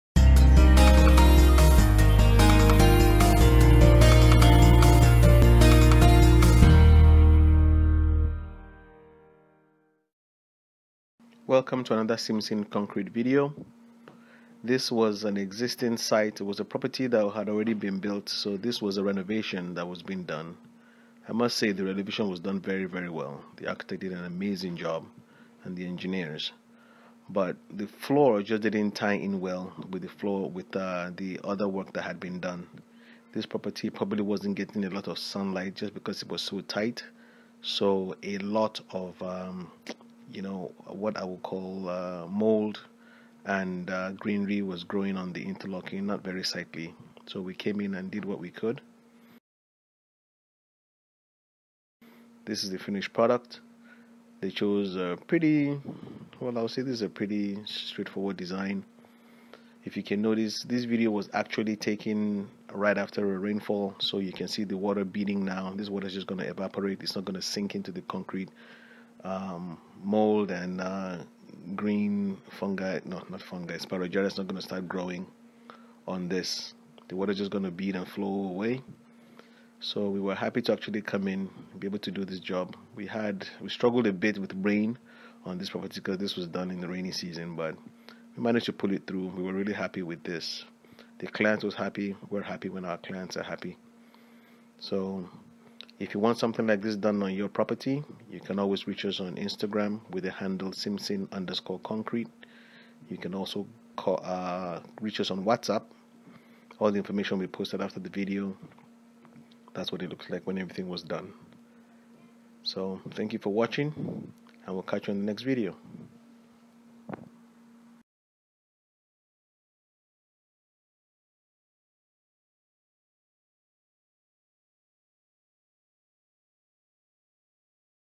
Concrete Engraving Kaduna Nigeria. Before and After...
Concrete engraving done in Kaduna Nigeria.